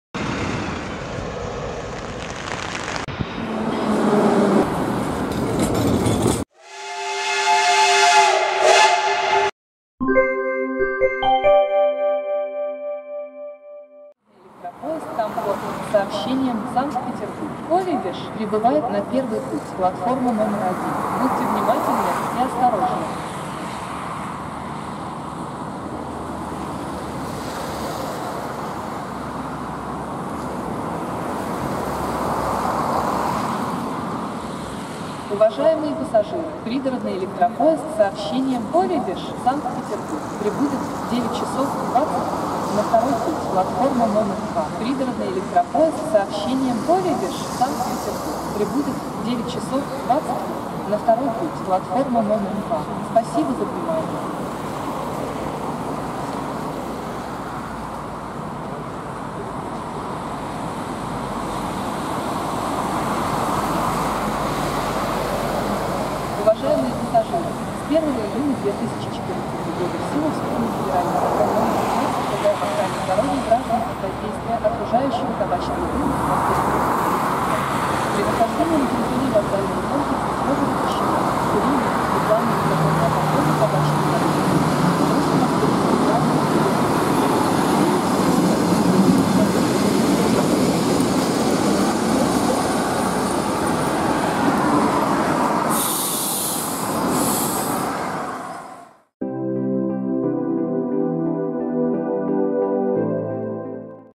"Морозным утром!" Электропоезд "Кософар" ЭД4М - 0412 прибывает на о.п.Проспект Славы
Электропоезд постоянного тока "Кософар" ЭД4М - 0412 (Приписан в электродепо ТЧ-15 "Санкт-Петербург-Балтийский")
Пригородный поезд "Комфорт" №6465,сообщением "Комфорт" -
"Витебский вокзал - Оредеж" прибывает на о.п."Проспект Славы".

Перегон "Санкт-Петербург-Товарный-Витебский - Шушары" Витебского направления Октябрьской Железной Дороги.ОктЖД,РЖД.
Санкт-Петербург,Россия.
13.XII.2020